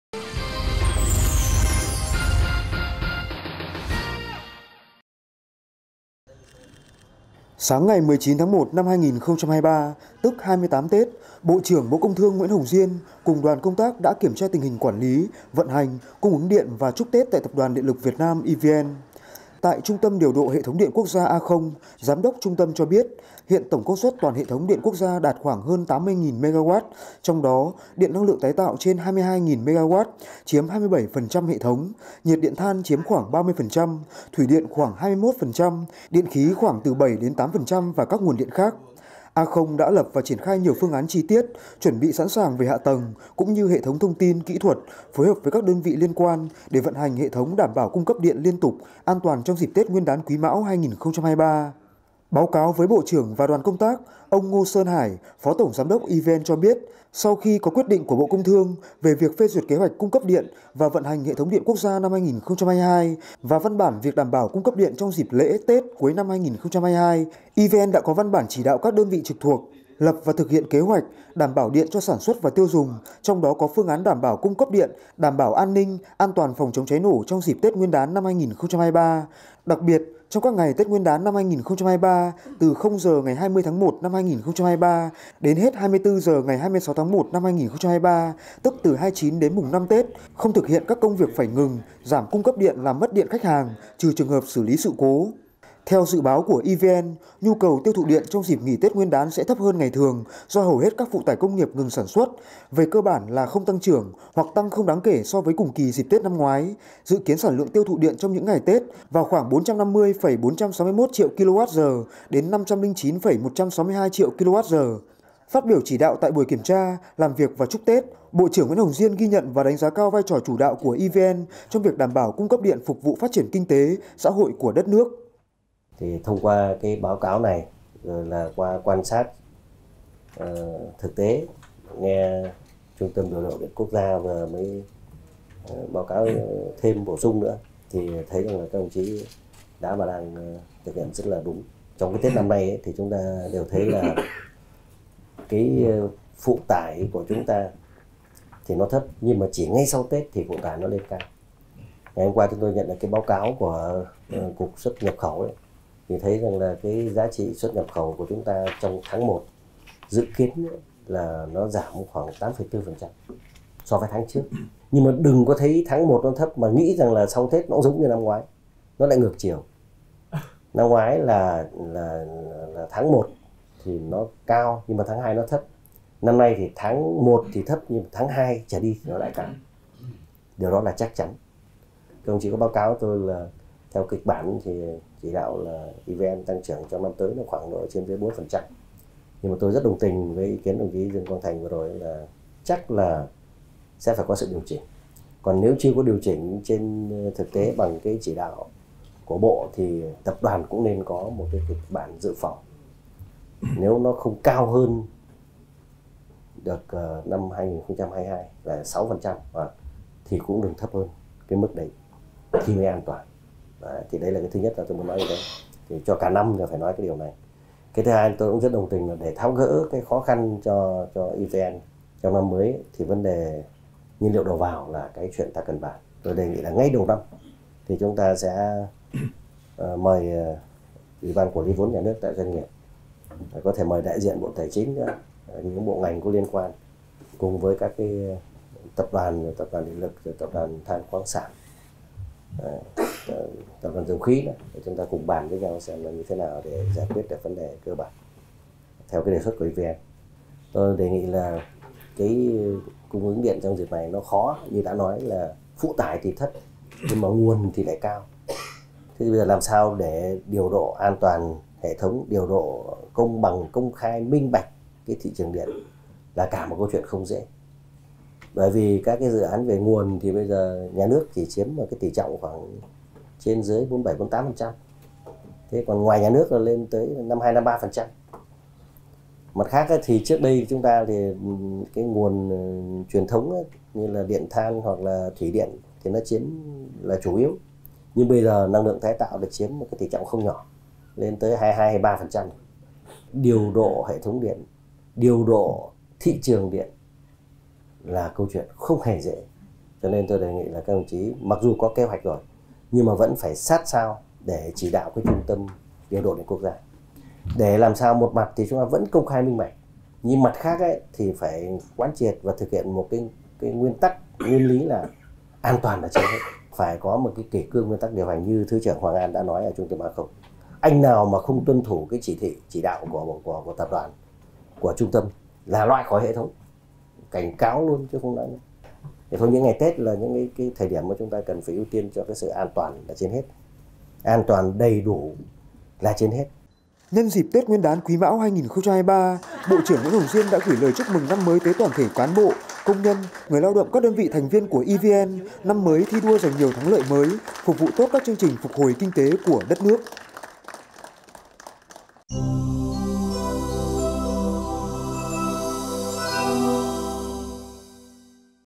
Bộ trưởng Bộ Công Thương kiểm tra an toàn phát điện dịp Tết Nguyên Đán
Sáng ngày 19/01/2023 (tức 28 Tết), Bộ trưởng Bộ Công Thương Nguyễn Hồng Diên cùng đoàn công tác đã kiểm tra tình hình quản lý, vận hành, cung ứng điện và chúc Tết tại Tập đoàn Điện lực Việt Nam (EVN)…